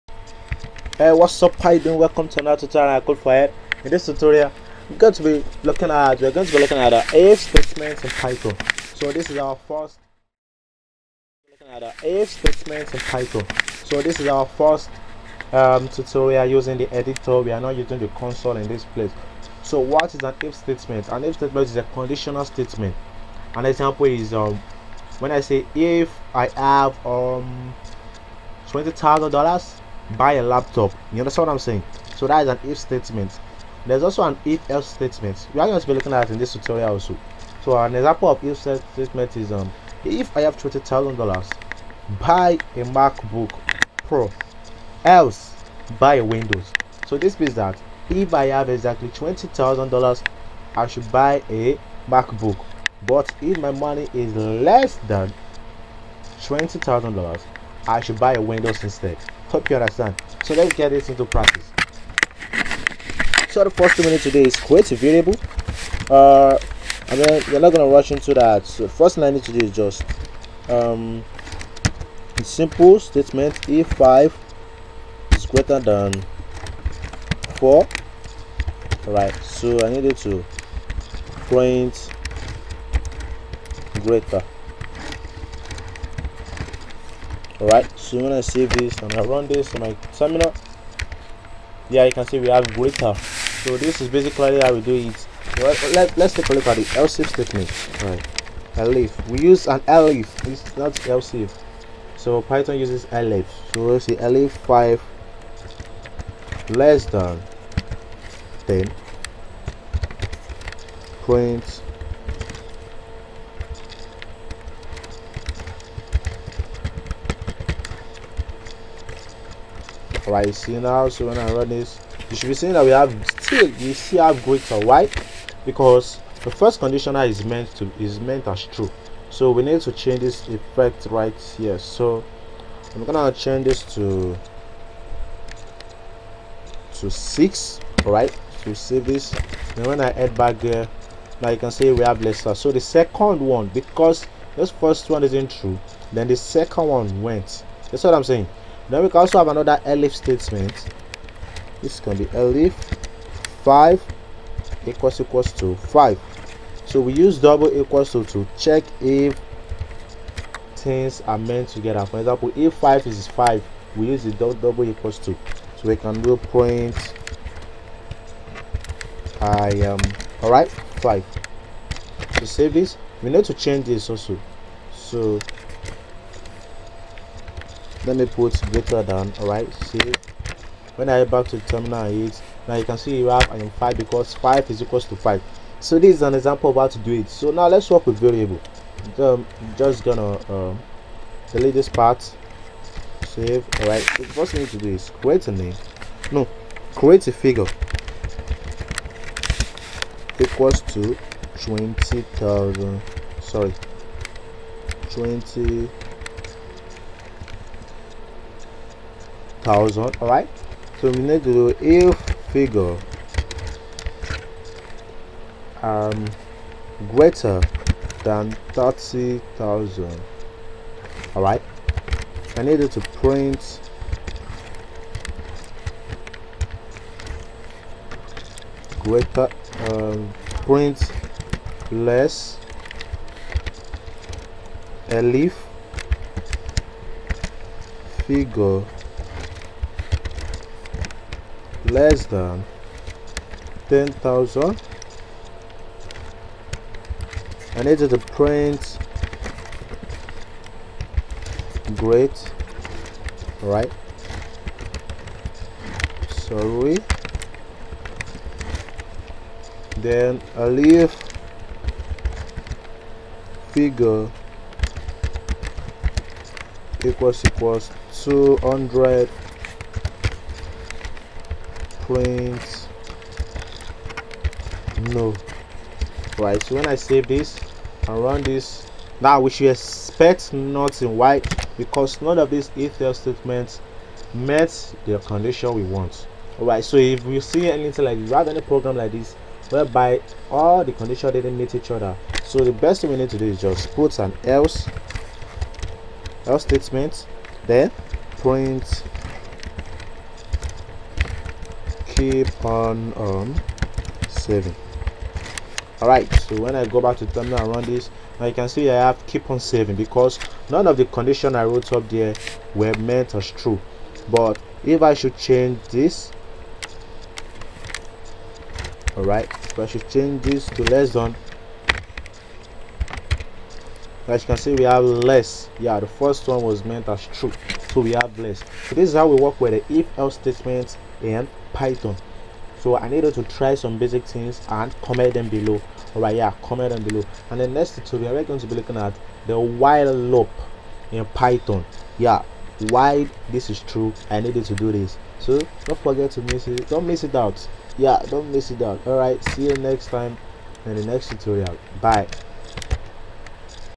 Python Tutorial - IF ELSE STATEMENT
In this tutorial, you will learn about if else statement in python